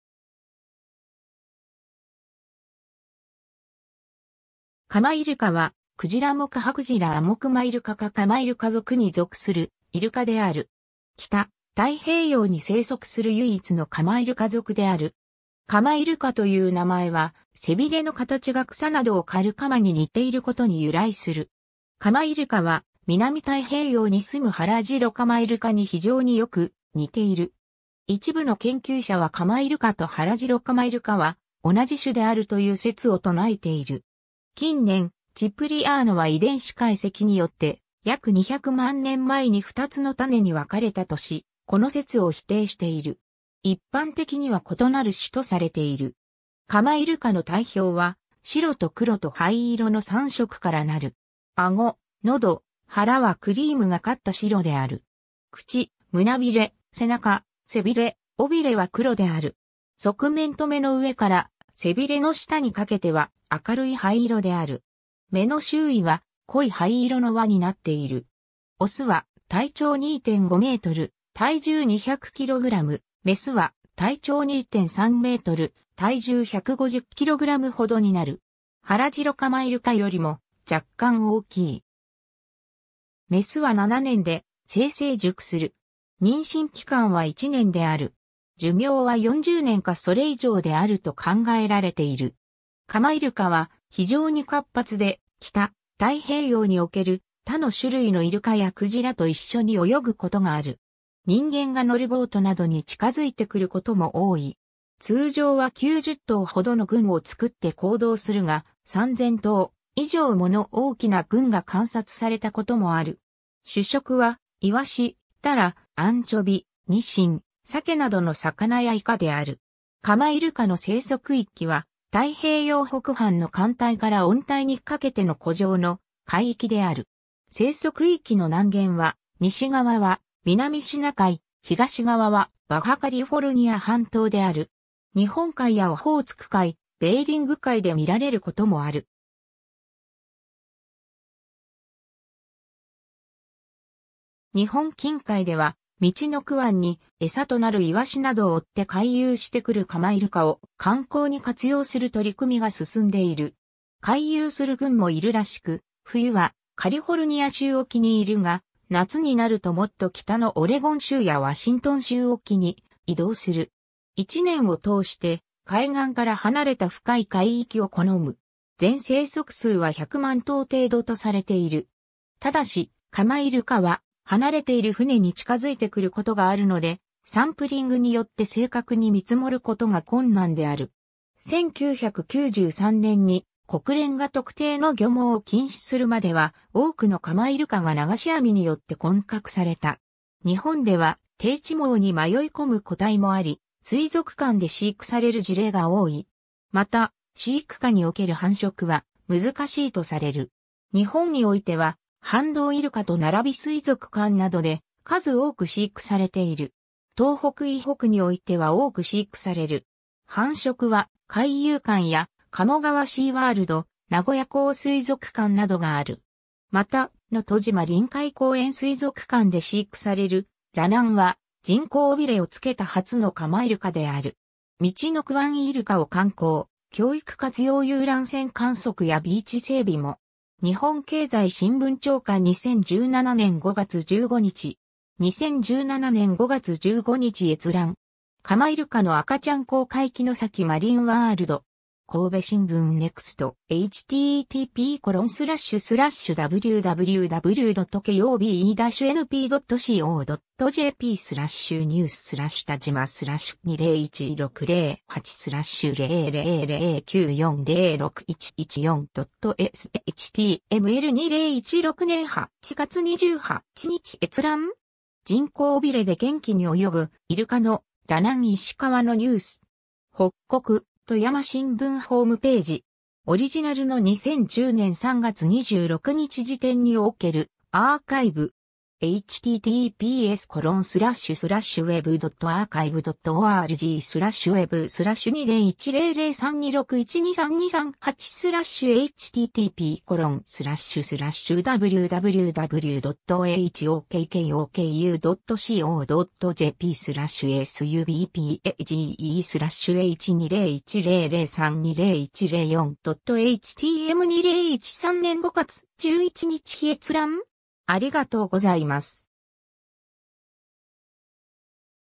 カマイルカ
カマイルカ（Lagenorhynchus obliquidens、鎌海豚）はクジラ目ハクジラ亜目マイルカ科カマイルカ属に属するイルカである。
北太平洋に棲息する唯一のカマイルカ属（Lagenorhynchus）である。
カマイルカという名前は、背びれの形が草などを刈る鎌に似ていることに由来する。
 カマイルカは、南太平洋に棲むハラジロカマイルカに非常に良く似ている。
一部の研究者はカマイルカとハラジロカマイルカは同じ種であるという説を唱えている。
近年、Ciprianoは遺伝子解析によって、約200万年前に二つの種に別れたとし、この説を否定している。
一般的には異なる種とされている。
 カマイルカの体表は白と黒と灰色の3色から成る。
顎、喉、腹はクリームがかった白である。
口、胸びれ、背中、背びれ、尾びれは黒である。
側面と眼の上から背びれの下にかけては明るい灰色である。
眼の周囲は濃い灰色の輪になっている。
 雄は体長2.5m、体重200kg、雌は体長2.3m、体重150kgほどになる。
ハラジロカマイルカよりも若干大きい。
雌は7年で性成熟する。
妊娠期間は1年である。
寿命は40年かそれ以上であると考えられている。
 カマイルカは非常に活発で、北太平洋における他の種類のイルカやクジラと一緒に泳ぐことがある。
人間が乗るボートなどに近づいてくることも多い。
通常は90頭ほどの群を作って行動するが、3000頭以上もの大きな群が観察されたこともある。
主食はイワシ、タラ、アンチョビ、ニシン、サケなどの魚やイカである。
 カマイルカの生息域は太平洋北半の寒帯から温帯にかけての弧状の海域である。
生息域の南限は、西側は南シナ海、東側はバハ・カリフォルニア半島である。
日本海やオホーツク海、ベーリング海で見られることもある。
日本近海では陸奥湾に餌となるイワシなどを追って回遊してくるカマイルカを観光に活用する取り組みが進んでいる。
 回遊する群もいるらしく、冬はカリフォルニア州沖にいるが、夏になるともっと北のオレゴン州やワシントン州沖に移動する。
1年を通して、海岸から離れた深い海域を好む。
 全生息数は100万頭程度とされている。
ただし、カマイルカは離れている船に近づいてくることがあるので、サンプリングによって正確に見積もることが困難である。
 1993年に国連が特定の魚網を禁止するまでは、多くのカマイルカが流し網によって混獲された。
日本では、定置網に迷い込む個体もあり、水族館で飼育される事例が多い。
また、飼育下における繁殖は難しいとされる（城崎マリンワールドで、2010・2015・2016年に成功した例がある）。
 日本においては、ハンドウイルカ（バンドウイルカ）と並び水族館などで数多く飼育されている。
東北以北においては多く飼育される。
繁殖は海遊館や鴨川シーワールド、名古屋港水族館などがある。
また、のとじま臨海公園水族館で飼育される「ラナン」は、人工尾びれを付けた初のカマイルカである。
 ^ “陸奥湾イルカを観光・教育活用 遊覧船観測やビーチ整備も”. 日本経済新聞朝刊2017年5月15日. 2017年5月15日閲覧。
^ “カマイルカの赤ちゃん公開　城崎マリンワールド”. 神戸新聞NEXT. (2016年8月20日). 2016年8月28日閲覧。
 ^ “人工尾びれで元気に泳ぐ イルカの「ラナン」 - 石川のニュース”. 北國・富山新聞ホームページ. (2010年3月20日03時04分更新). オリジナルの2010年3月26日時点におけるアーカイブ。
2013年5月11日閲覧。